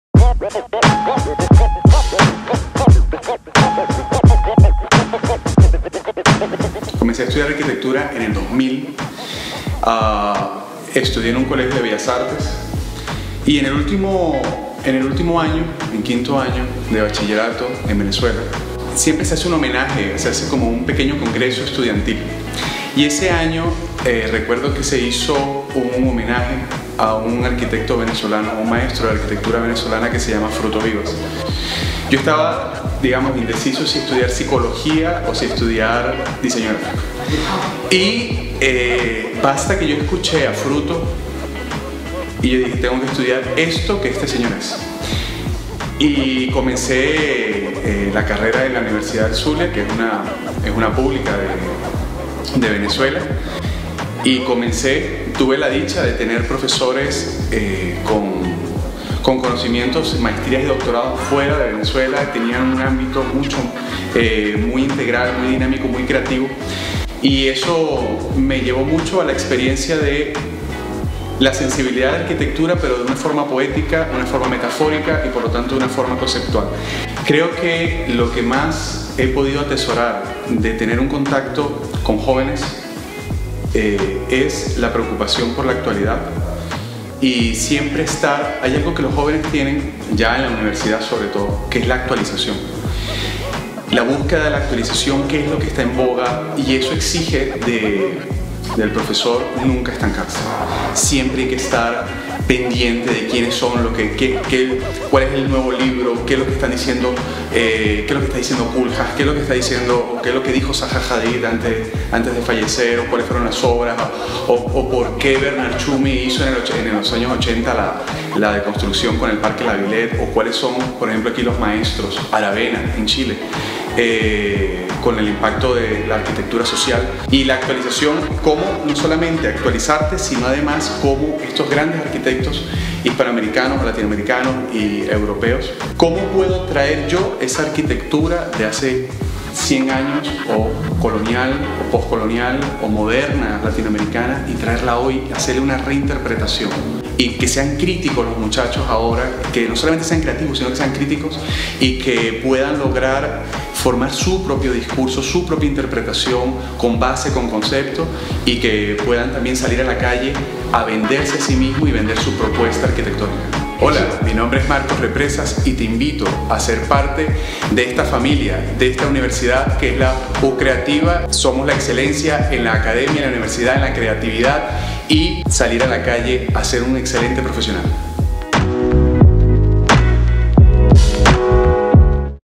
¡Conoce a tu profe Marcos Represas!
🤓 Marcos es uno de nuestros profes de la Escuela de Arquitectura y Diseño Interior. Es arquitecto y magister en filosofía, además cuenta con más de una docena de exposiciones de pintura. Ha trabajado en construcción y diseño urbano en Venezuela, en un despacho legislativo en la Asamblea de Costa Rica, tiene una revista regional en la Zona Norte llamada Gente Tuanis y da clases de filosofía. Marcos también es youtuber y da clases de edición profesional de video y YouTube en la Maestría de Marketing Digital de la Quality Leadership University (Panamá).
¡Conocé más de Marcos en la U! Siempre estará dispuesto a brindarte los mejores consejos y tips para tus proyectos más creativos. ✨